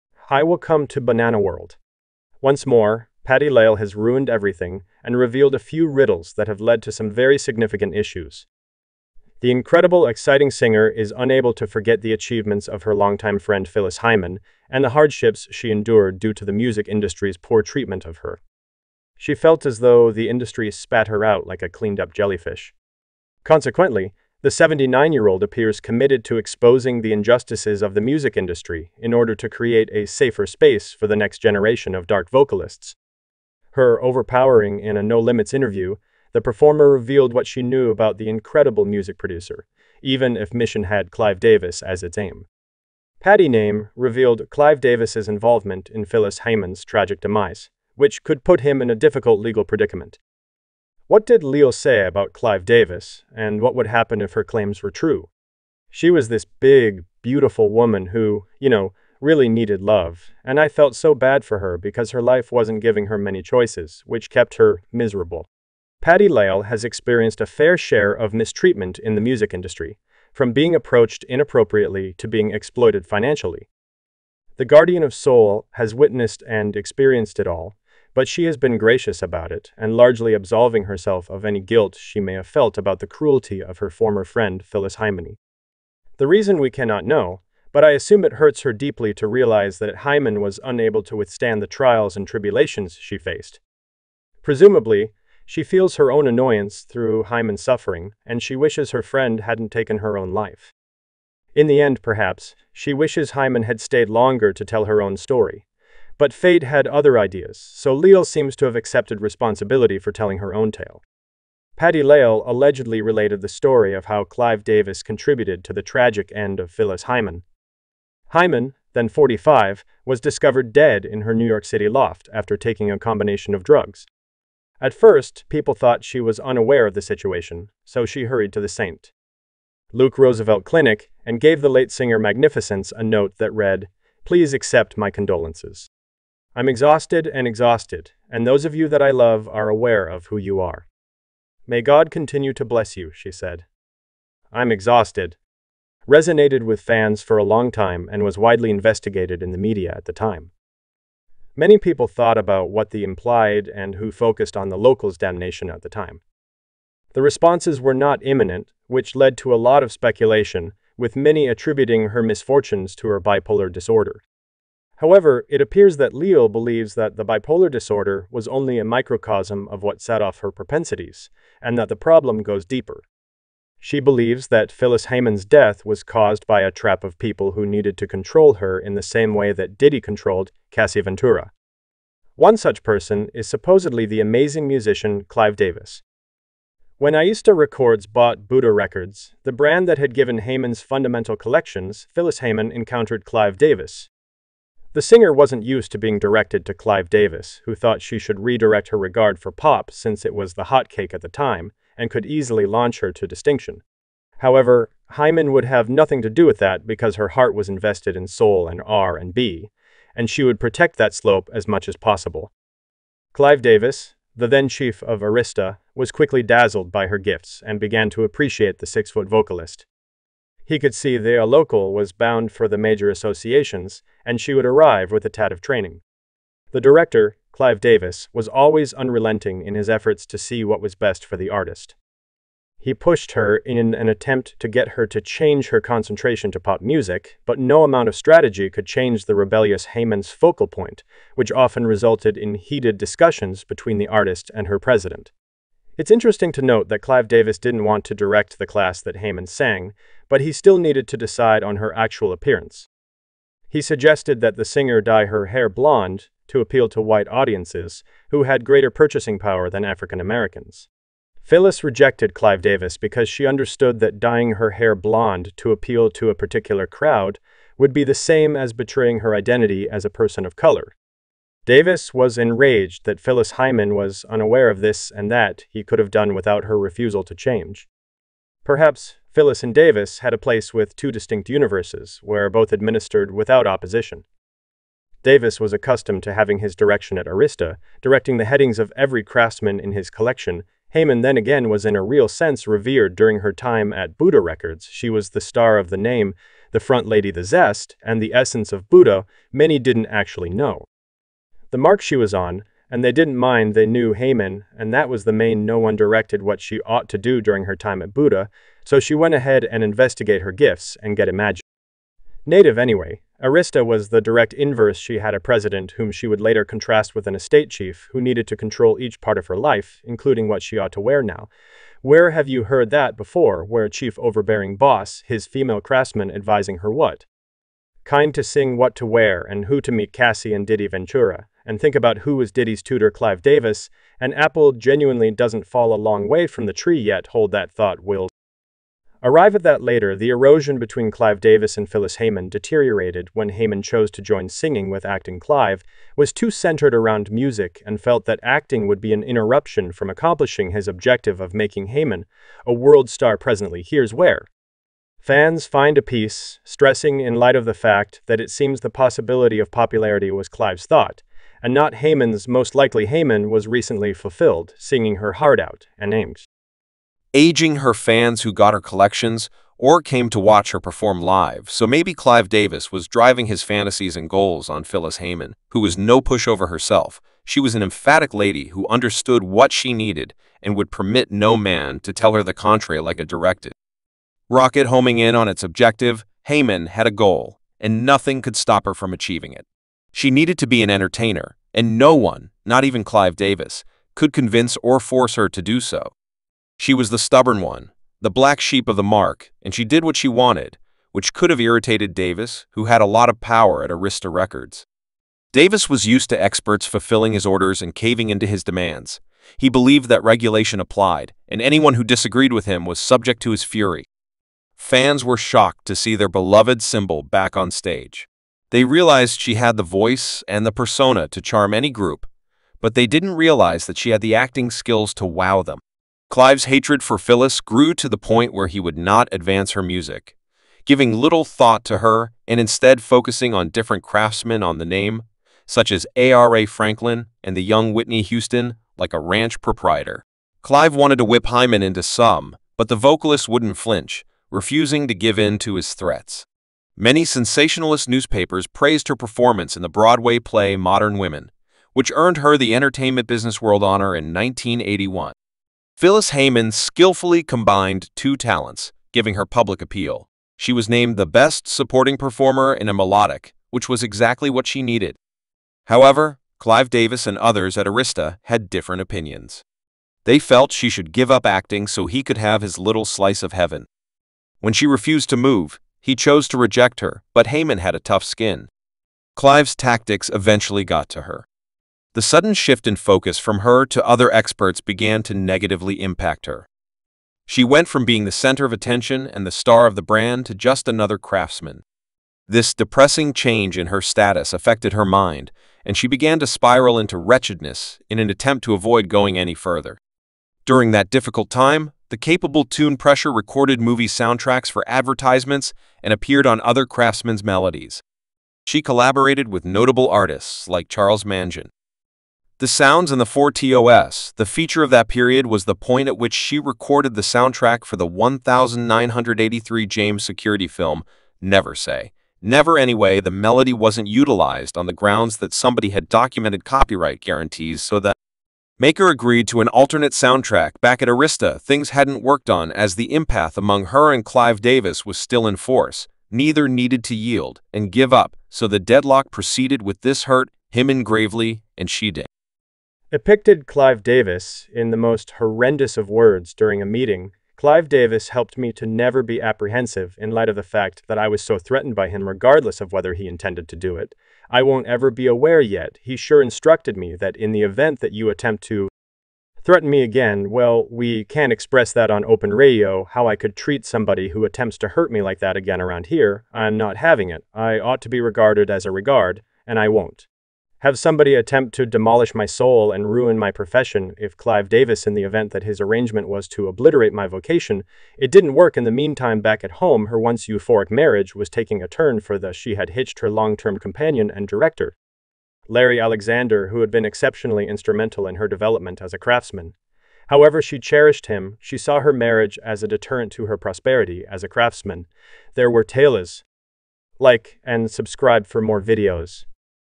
"Patti Labelle EXPOSES Clive Davis's Role in Phyllis Hyman's Tragic End | Shocking Revelations!"
"In a shocking revelation, Patti Labelle exposes Clive Davis's involvement in the tragic end of the talented singer Phyllis Hyman. Discover the untold story behind the scenes and how industry pressures and personal struggles led to this heartbreaking

tragedy. Patti Labelle's candid account sheds light on the darker side of the music industry and the impact it had on Phyllis Hyman's life and career.

Don't miss out on this exclusive insight into the hidden truths of the entertainment world.